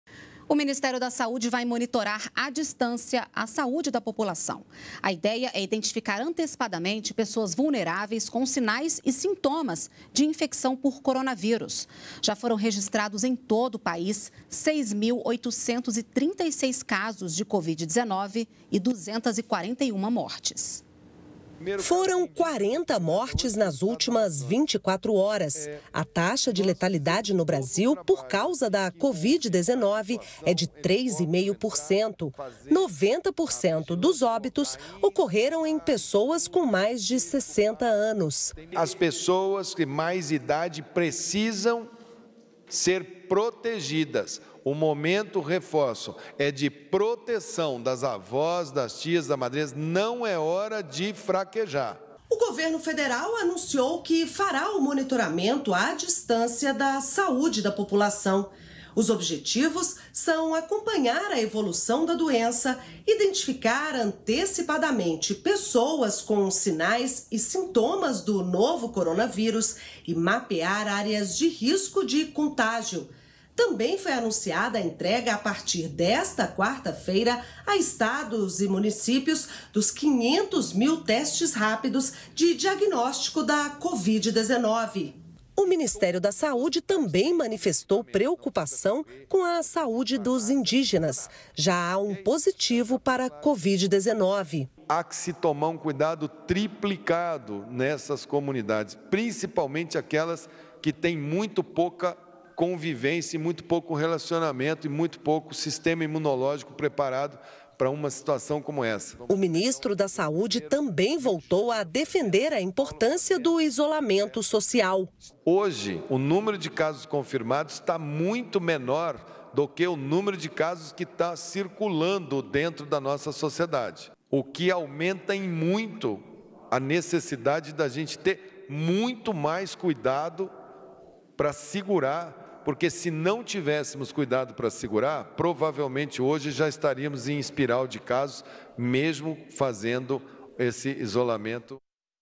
Brasil tem quase 7 mil casos de Covid-19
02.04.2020 - O Ministério da Saúde vai monitor a distância da saúde da população. A ideia é identificar antecipadamente pessoas vulneráveis, com sinais e sintomas de infecção por coronavírus. Já foram registrados, em todo o país, 6.836 casos de Covid-19 e 241 mortes.